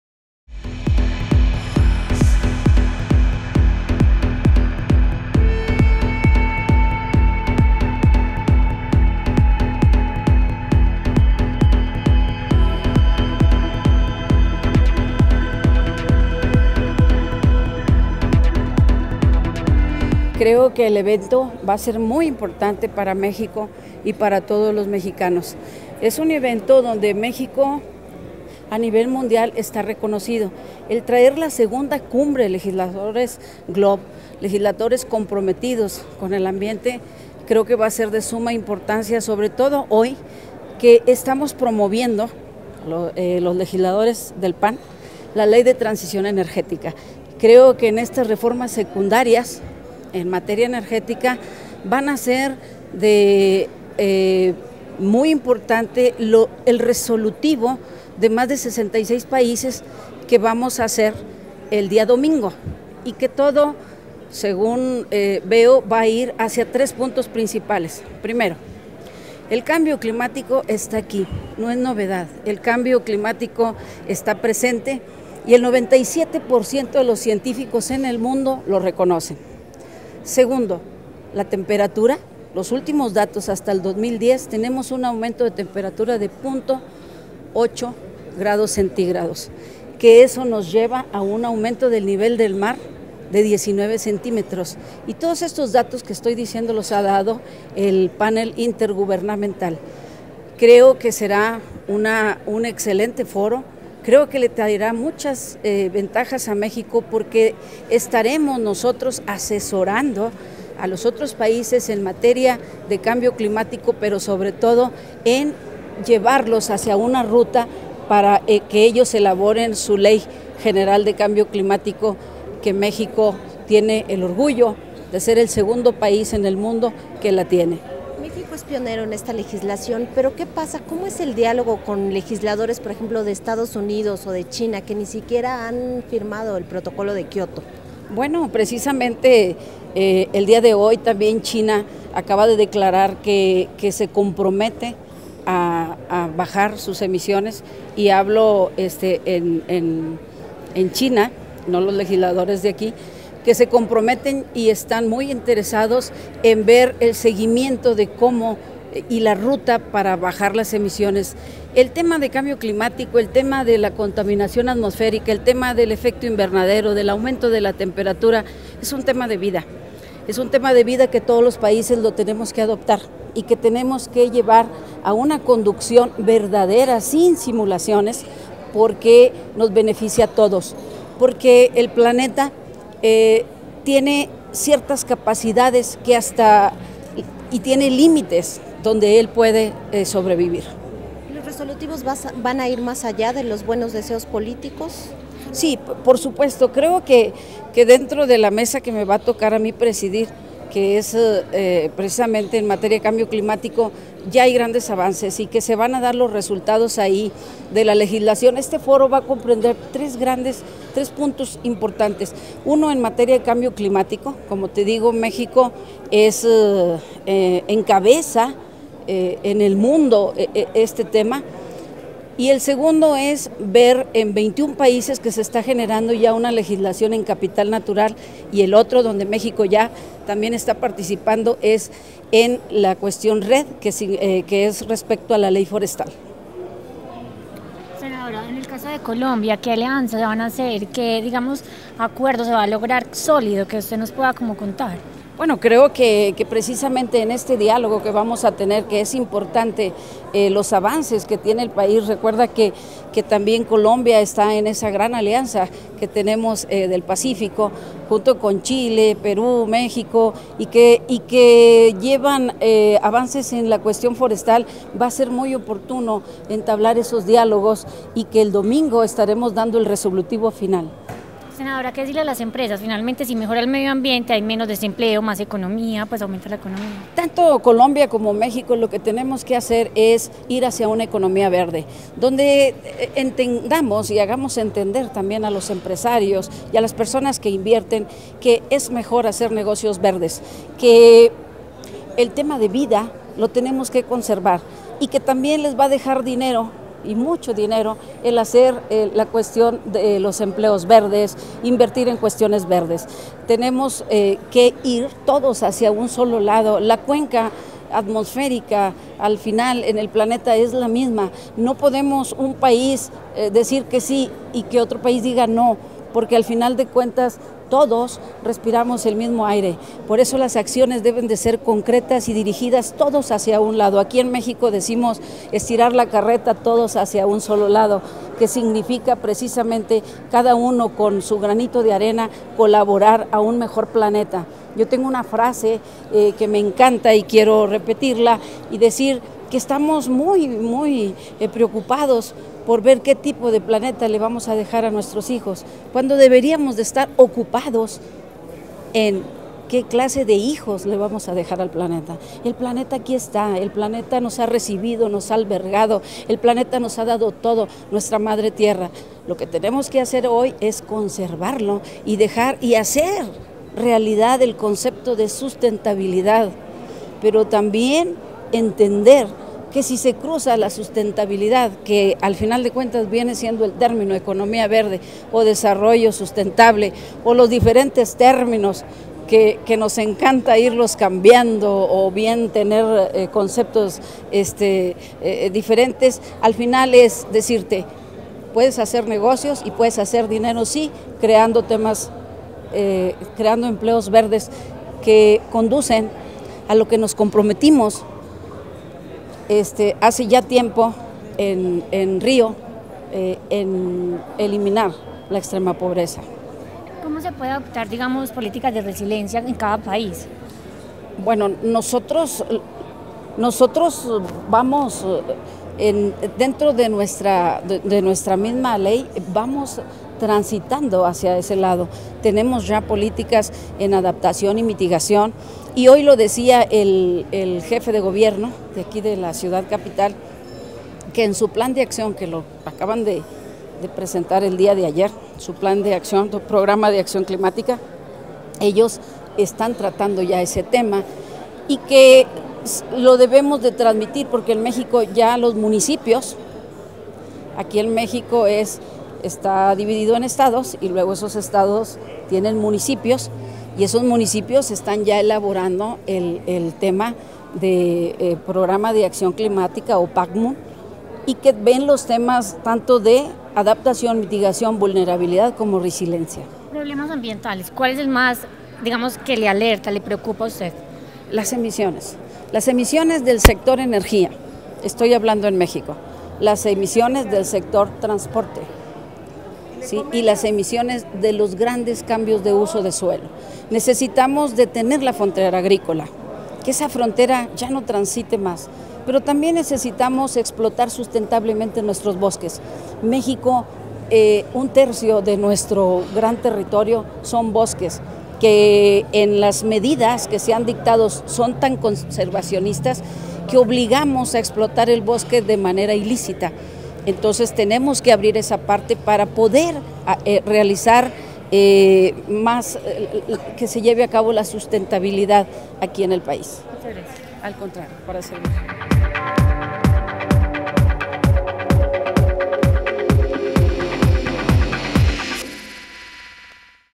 El Cambio Climático es un tema que todos los países deben adoptar: Sen. Garza Galván - 060614
México, D.F., 06 de junio de 2014.- Entrevista efectuada a la senadora Silvia Garza Galván, durante la inauguración de la Segunda Cumbre Mundial de Legisladores (GLOBE) en San Lázaro.